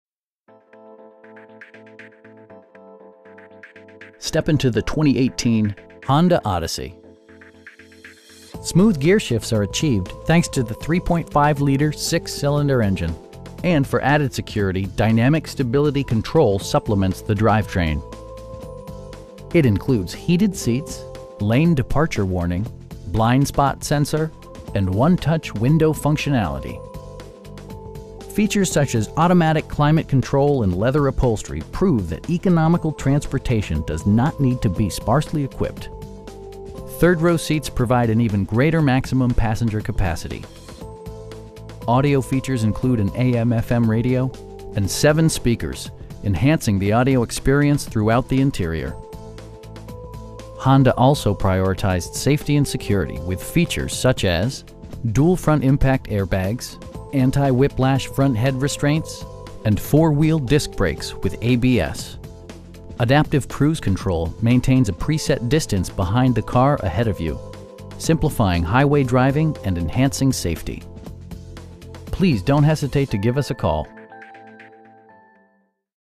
2018 Honda Odyssey EX-L in Panama City, FL 32404
Honda of Bay County

Step into the 2018 Honda Odyssey. Smooth gearshifts are achieved thanks to the 3.5 liter 6 cylinder engine, and for added security, dynamic Stability Control supplements the drivetrain. It includes heated seats, front and rear reading lights, speed sensitive wipers, a power seat, an automatic dimming rear-view mirror, lane departure warning, blind spot sensor, and 1-touch window functionality. Features such as automatic climate control and leather upholstery prove that economical transportation does not need to be sparsely equipped. Storage solutions are integrated throughout the interior, demonstrating thoughtful attention to detail. Third row seats provide an even greater maximum passenger capacity. For drivers who enjoy the natural environment, a power moon roof allows an infusion of fresh air. Audio features include an AM/FM radio, steering wheel mounted audio controls, and 7 speakers, providing excellent sound throughout the cabin. Honda also prioritized safety and security with features such as: dual front impact airbags, front side impact airbags, traction control, brake assist, anti-whiplash front head restraints, a security system, and 4 wheel disc brakes with ABS. Adaptive cruise control maintains a pre set distance behind the car ahead of you, simplifying highway driving and enhancing safety. You will have a pleasant shopping experience that is fun, informative, and never high pressured. Please don't hesitate to give us a call.